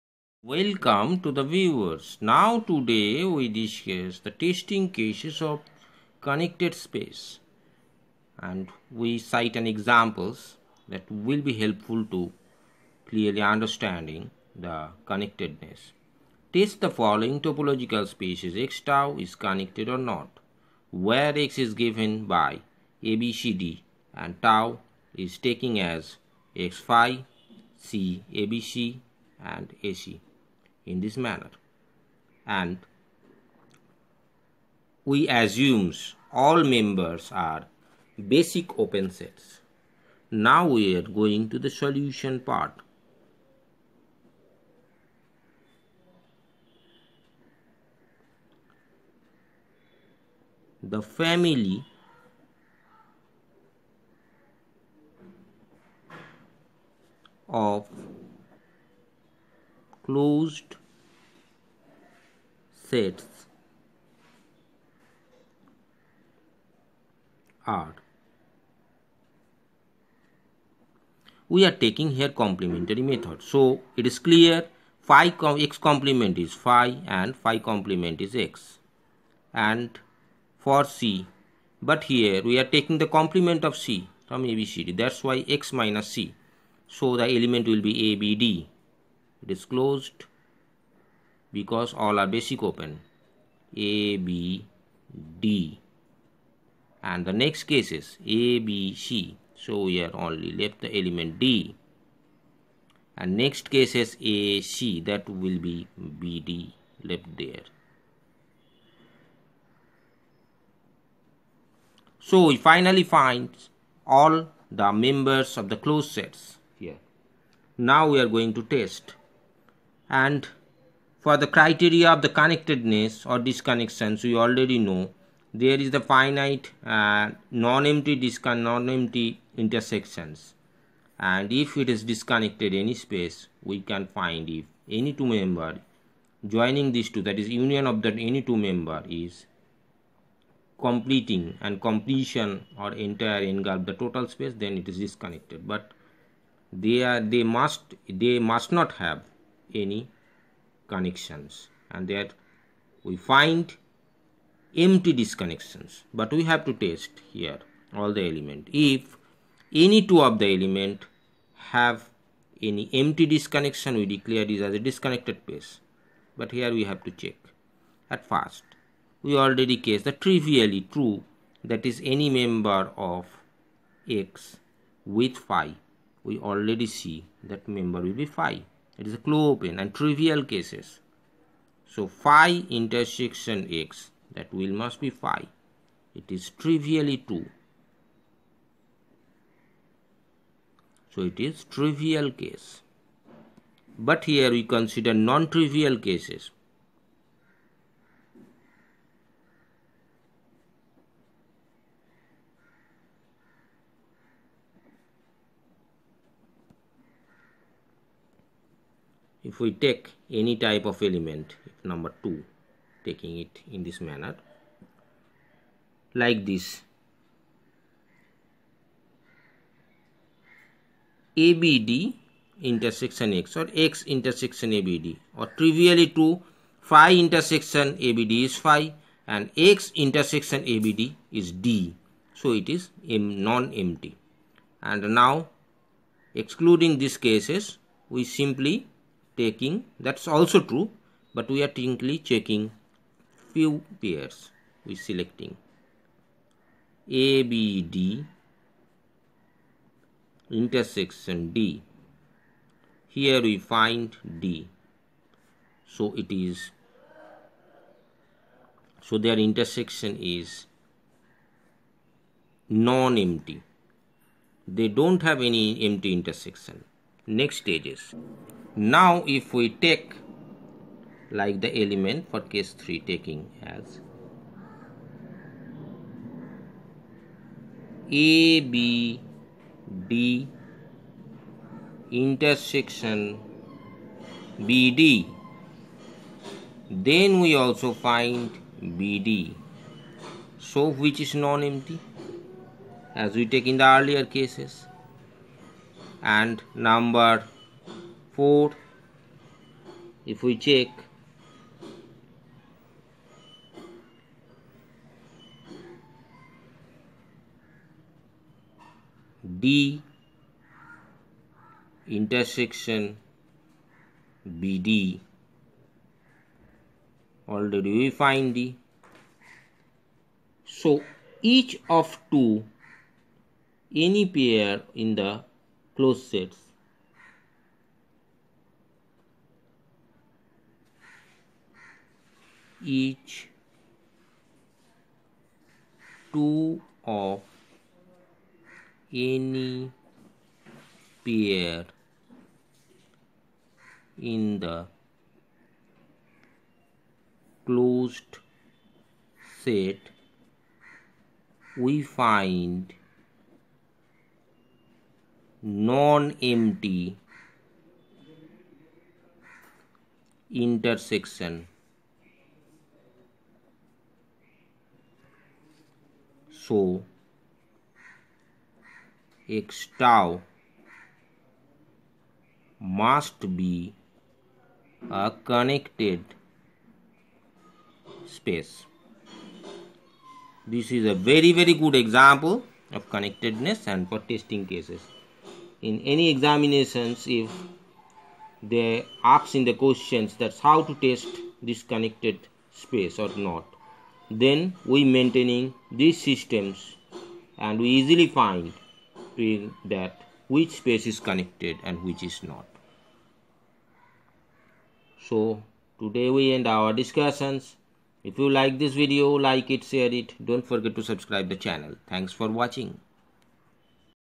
In general topology for mathematics| an important example of connectedness or connected space
Anyone may take each two distinct pair of basic open sets and proceed same way get same result.